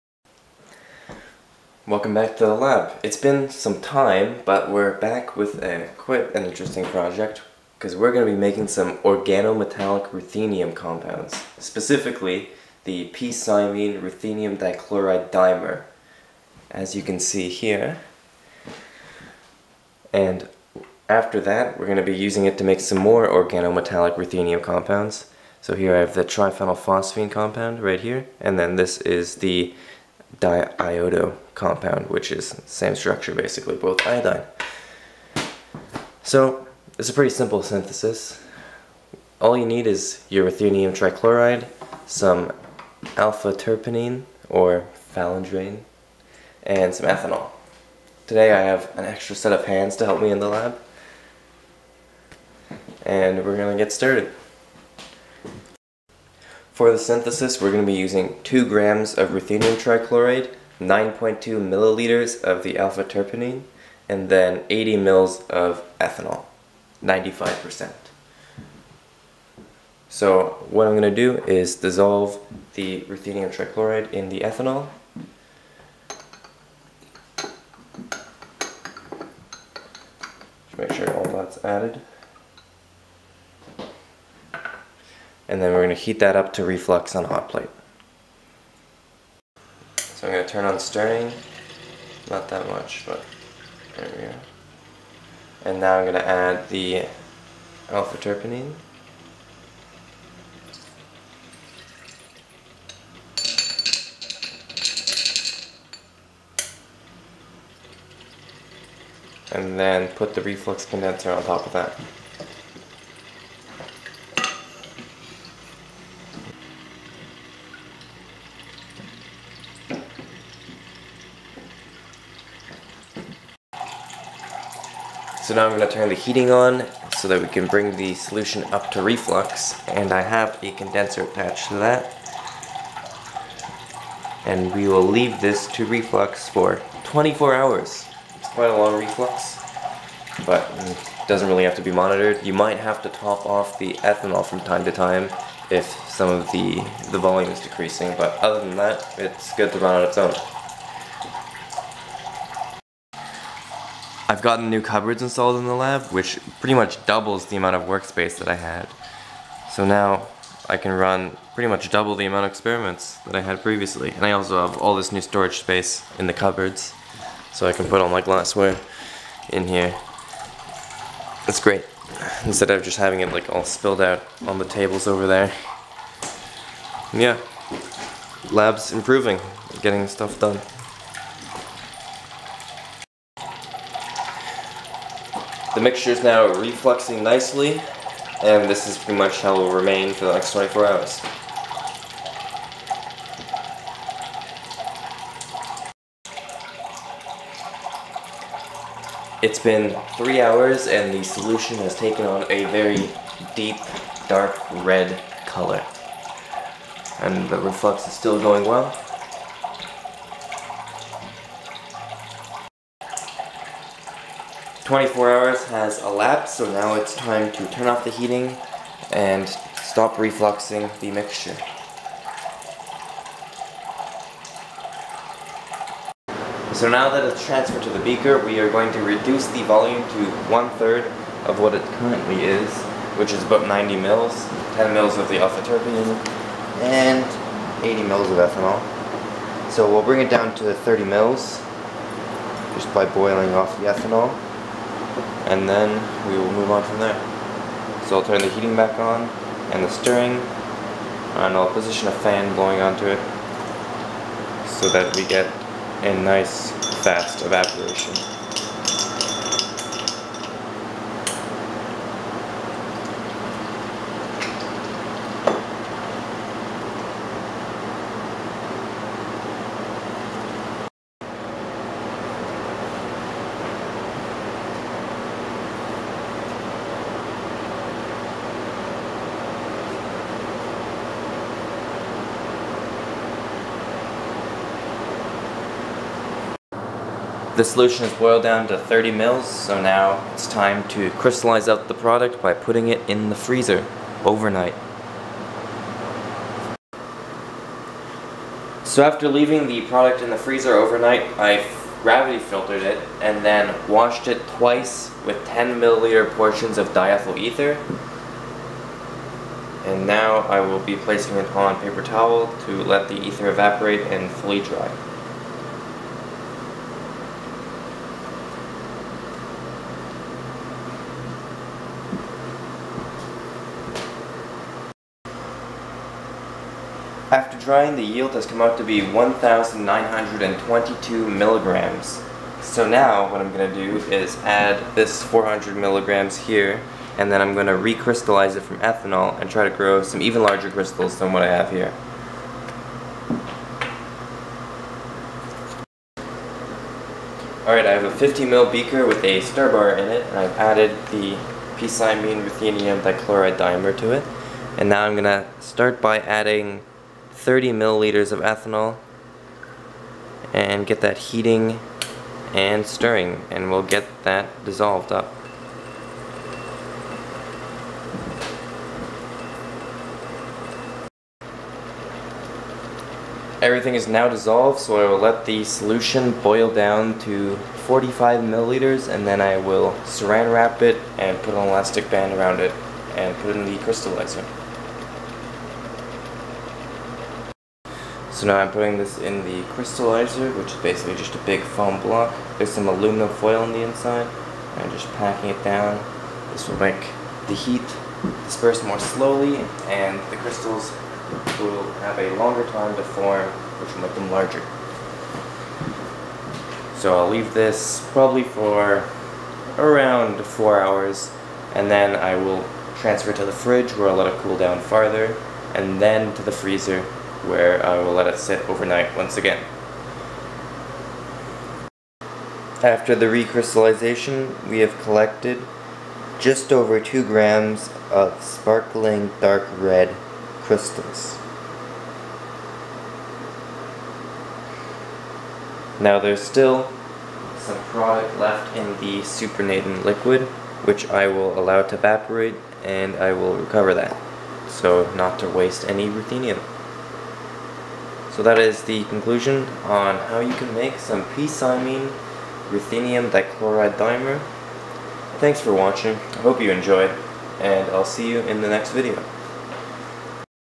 Making an Organometallic Ruthenium Complex (p-Cymene Ruthenium Dichloride Dimer)
In this video I made some pretty sparkling crystals of the p-cymene ruthenium dichloride dimer. I also got myself an upgraded lab bench and a new lab partner too!